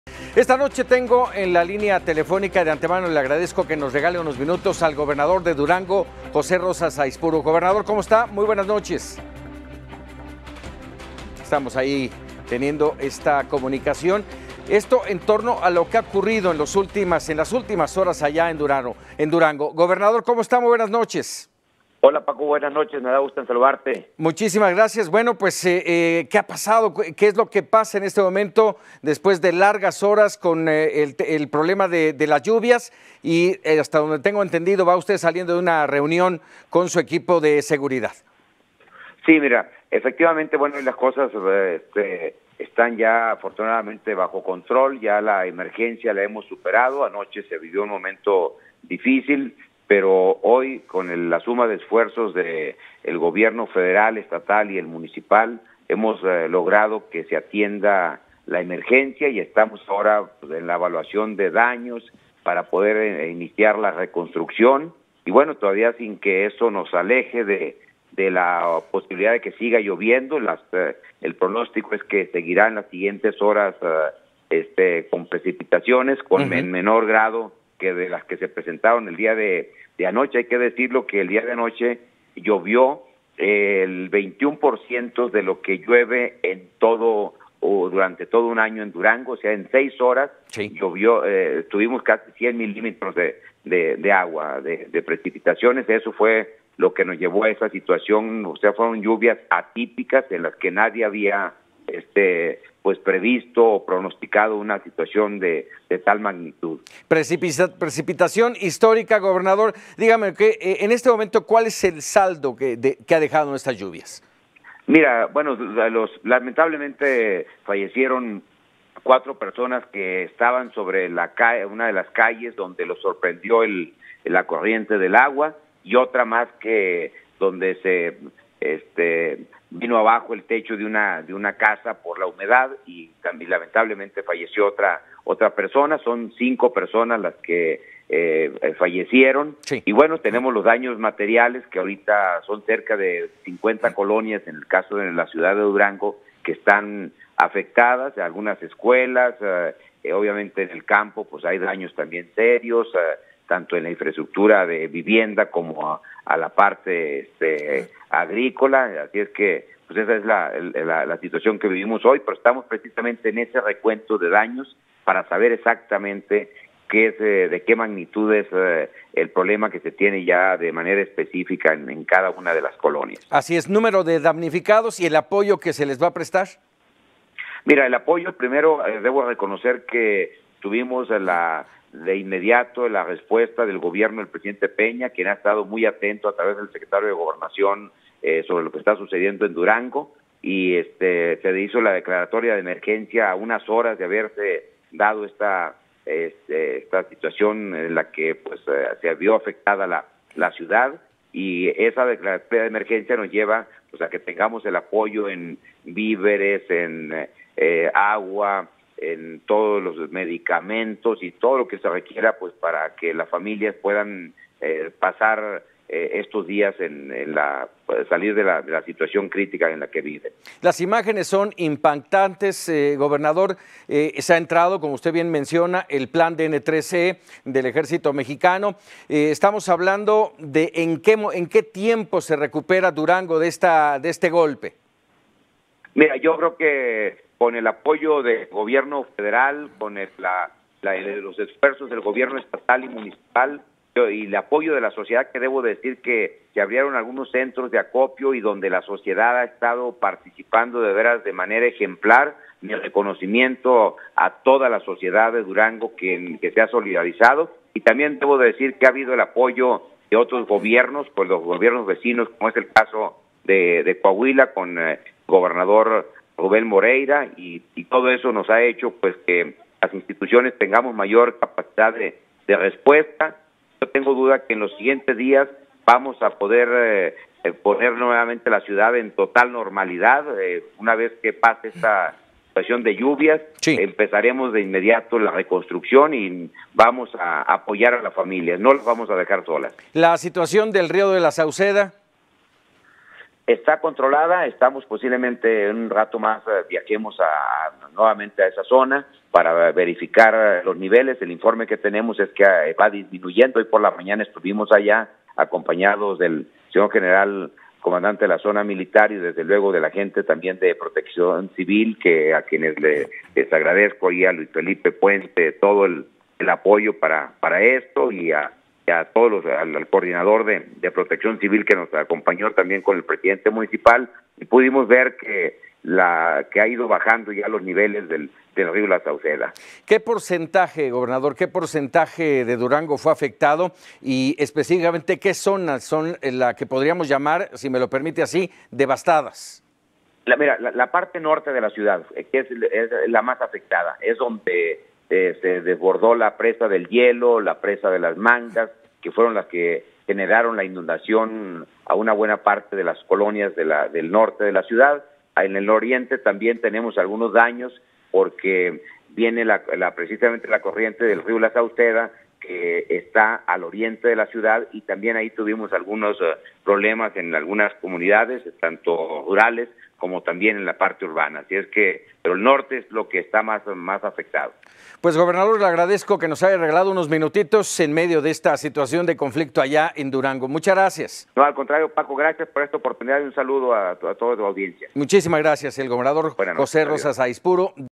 Entrevista a José Rosas Aispuro sobre emergencia por lluvias en Durango
Paco Ramírez habla con el gobernador de Durango.